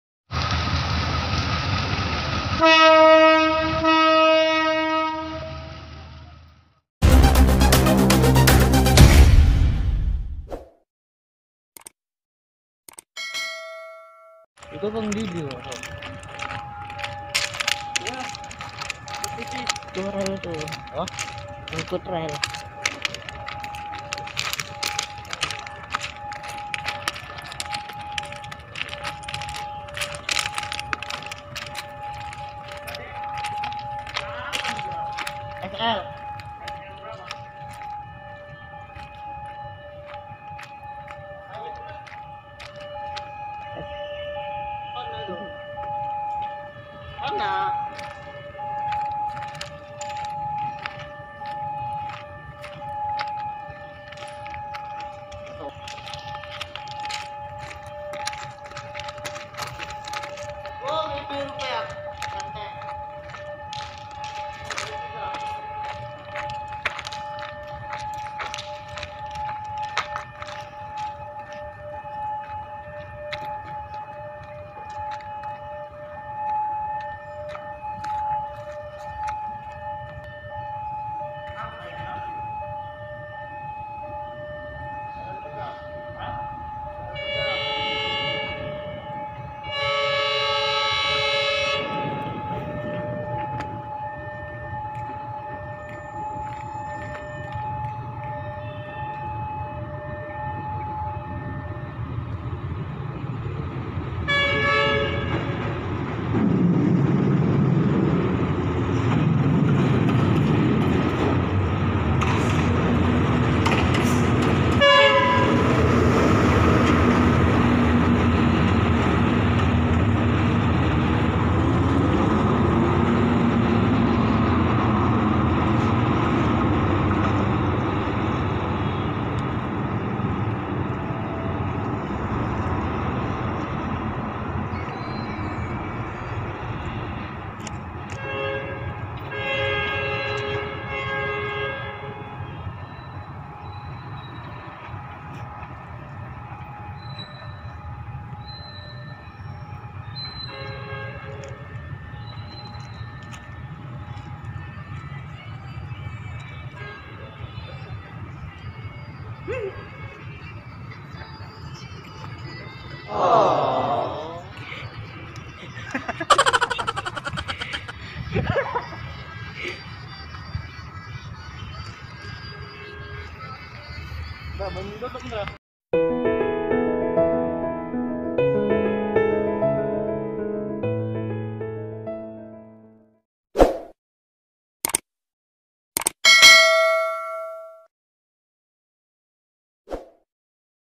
LL3 Berangkat Dari Stasiun Medan
LL3 Lokomotif BB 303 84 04 Berangkat dari Stasiun Medan

Semoga Kalian suka dengan video nya